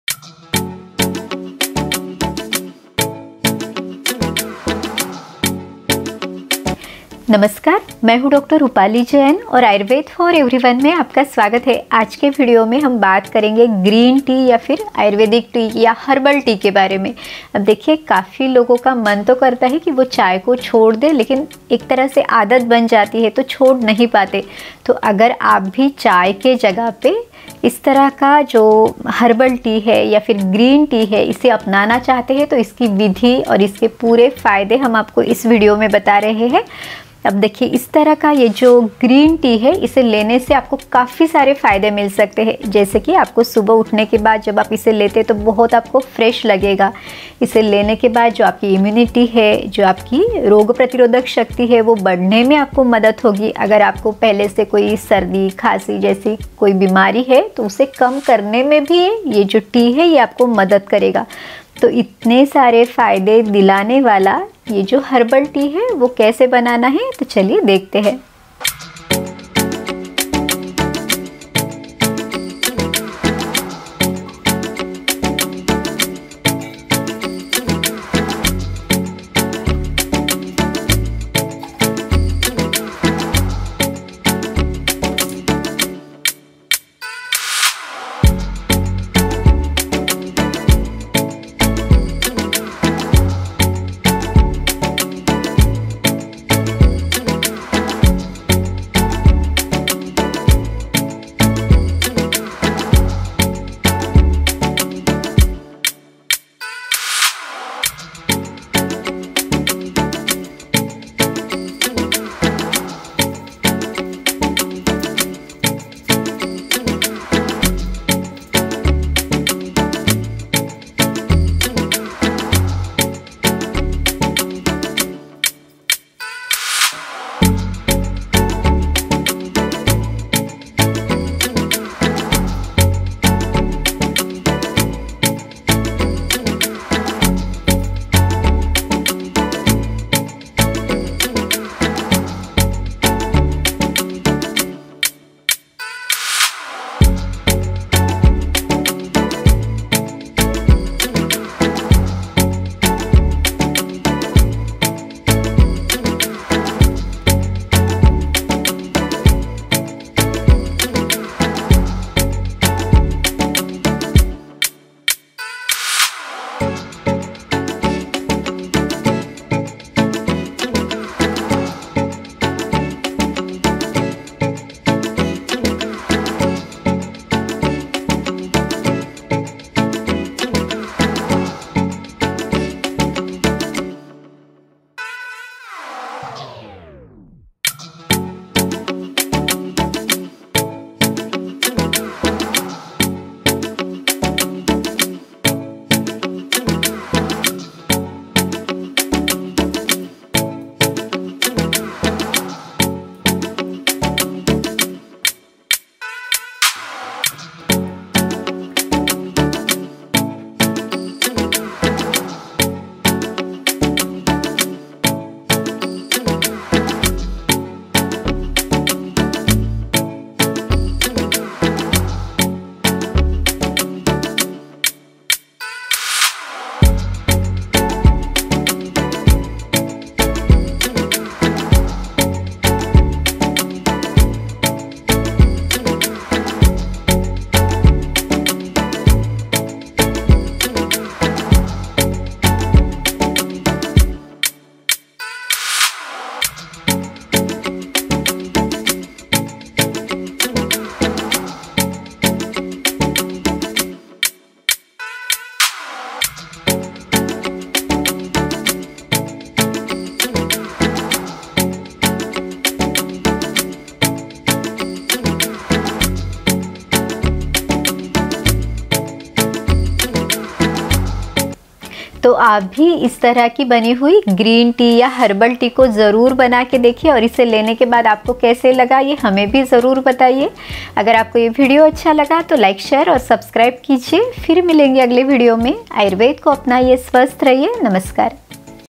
Best Green Tea with Amazing Health Benefits | Excellent for Weight loss, Fatty leaver & Immunity
How to make Ayurvedic Green Tea & it's Amazing Benefits